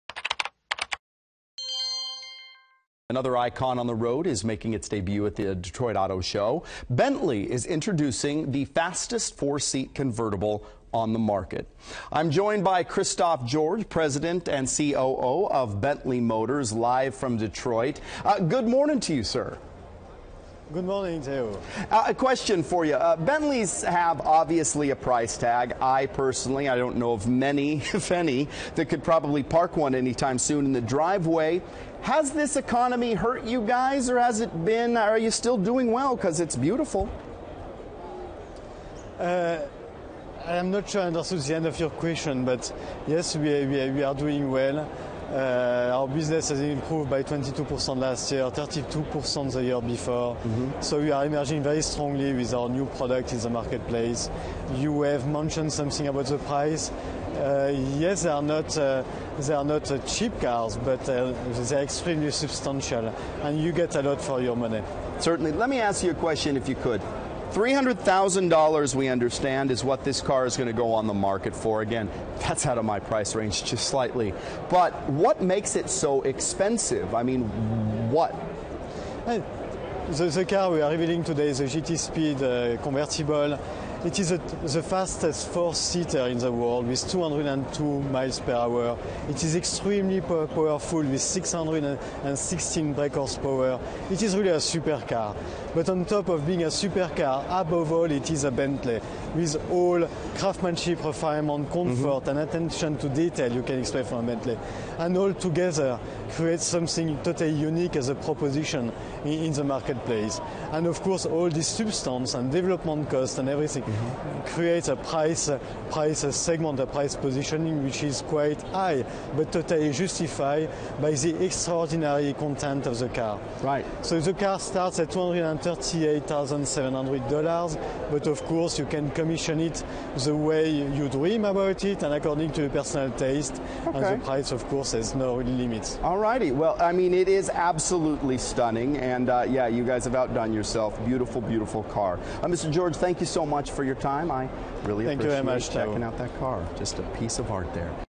Bentley rolls out $300K car at show
Christophe Georges, president of Bentley Motors, discusses the car company's latest model, which claims to be the fastest four-seater on the market.